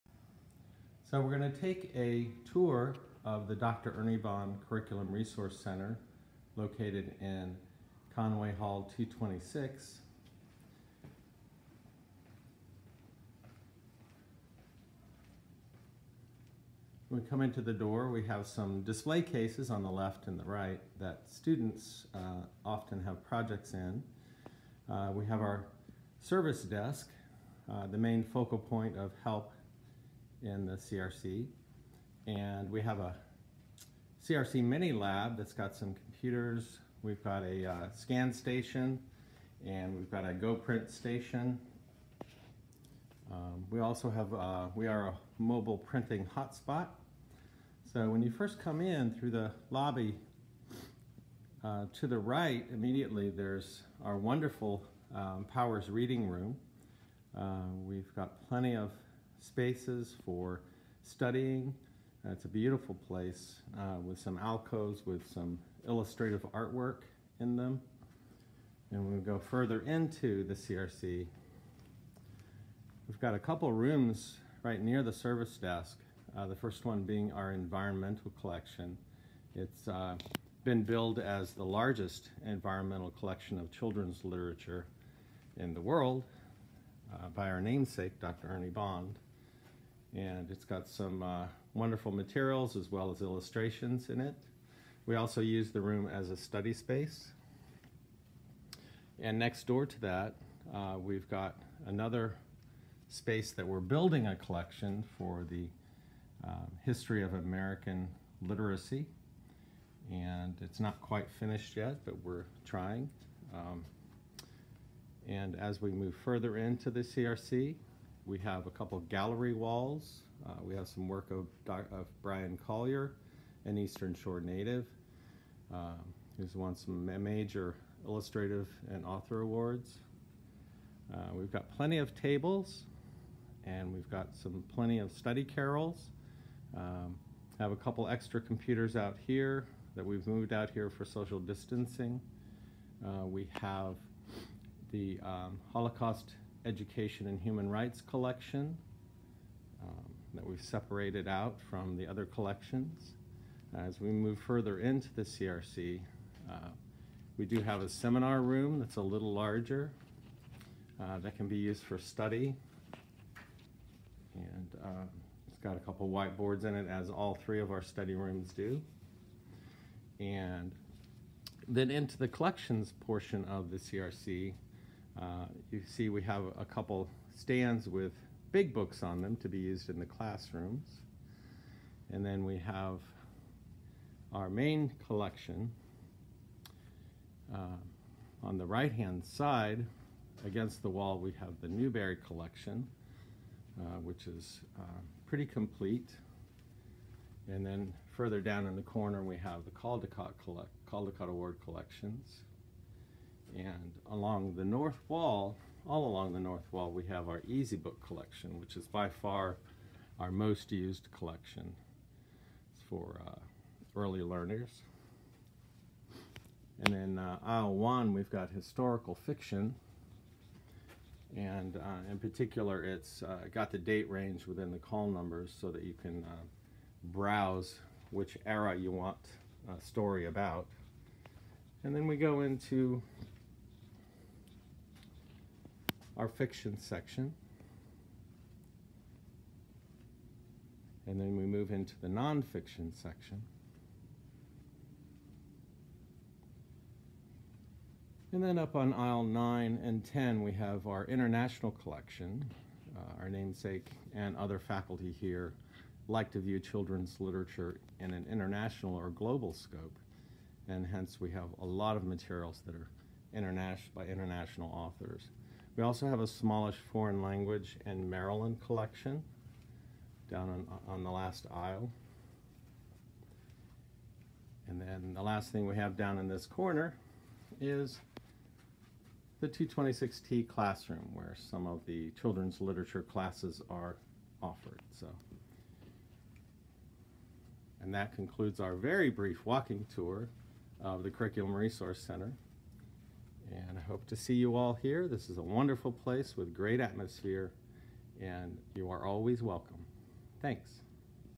Dr Ernie Bond Curriculum Resource Center Tour
This is a very brief 5 minute walk-through of the CRC, one of the SU Libraries, located in Conway Hall 226.